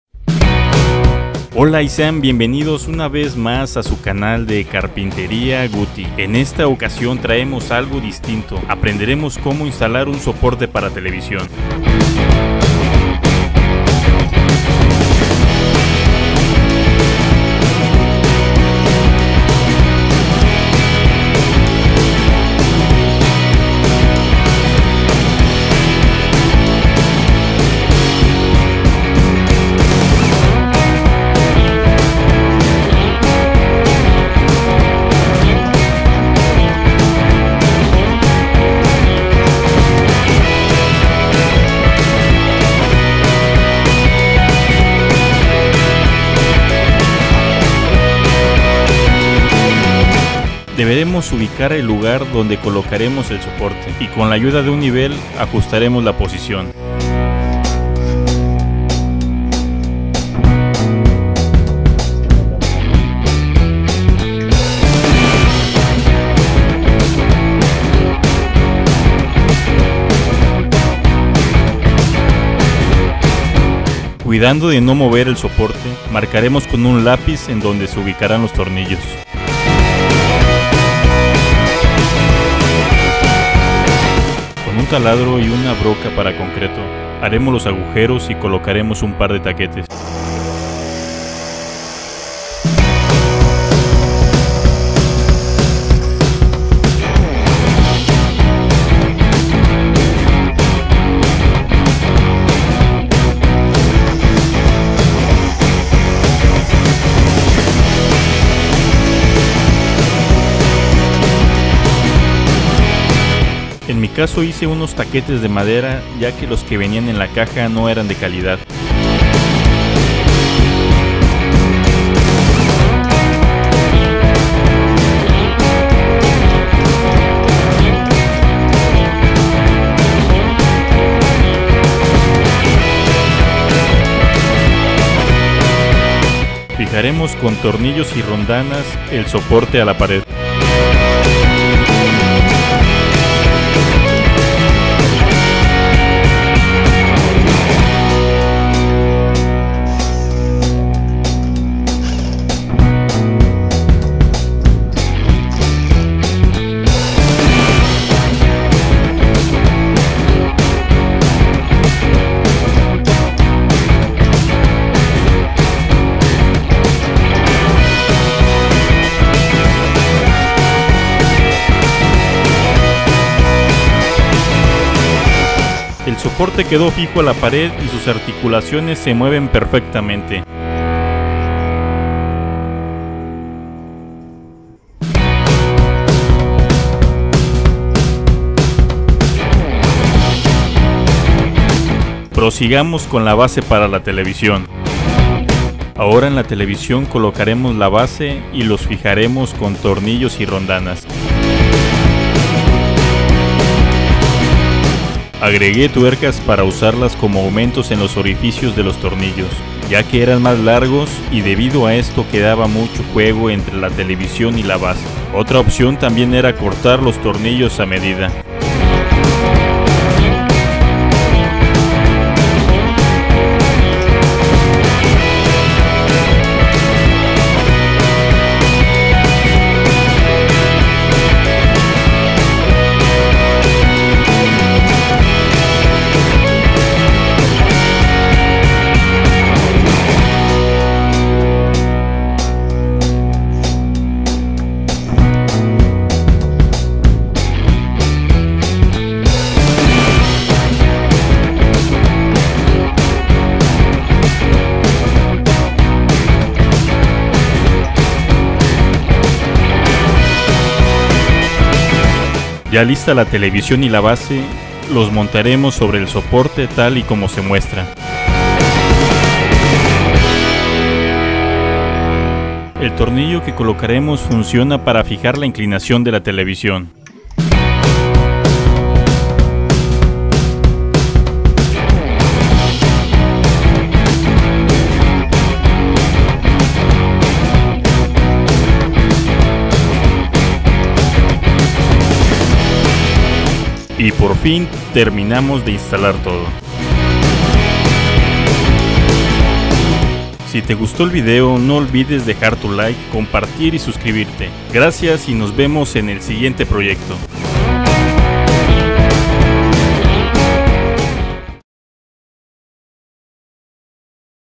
Como instalar un soporte de tv / How to install a tv stand
Hola amigos, traemos para ustedes como instalar un soporte para televisión,  un soporte de tv te ayuda mucho para ahorrar espacio, este tipo de brazo para televisión es muy sencillo de instalar.